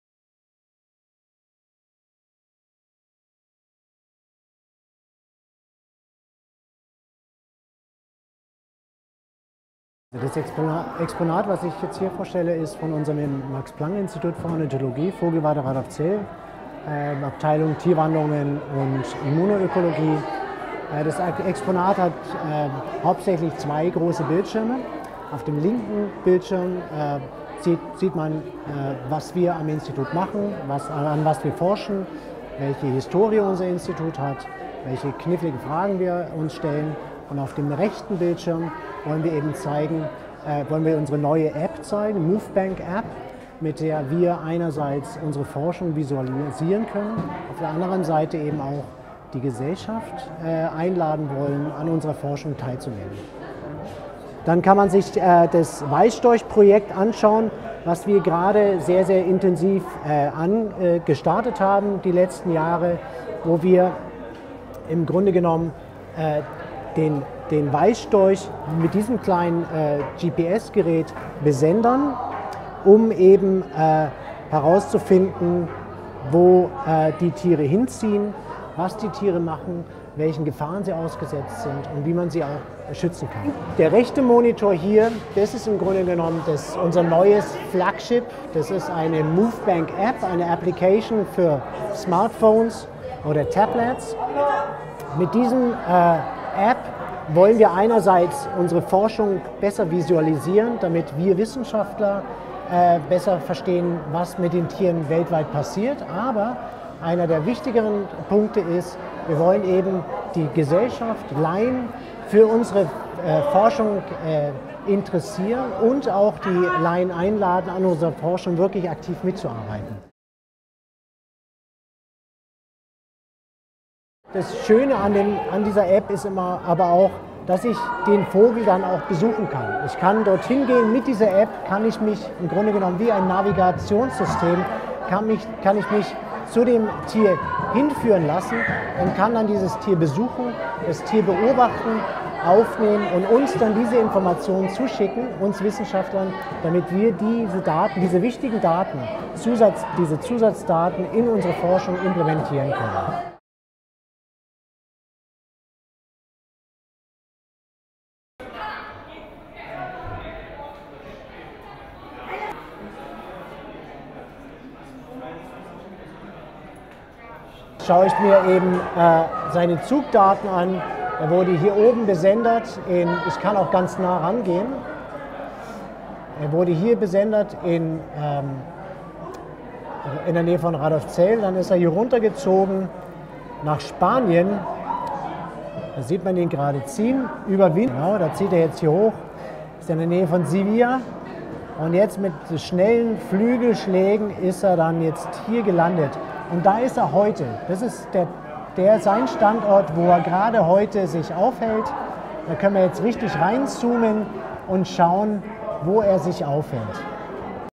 Max-Planck-Wissenschaftler über die Movebank-App
Dr. Jesko Partecke, Wissenschaftler am Max-Planck-Institut für Ornithologie, stellt das Exponat „Storchenflug mit Smartphone" und die „Movebank-App" vor, mit deren Hilfe Ausstellungsbesucher die Wanderung von Tieren fast in Echtzeit beobachten können.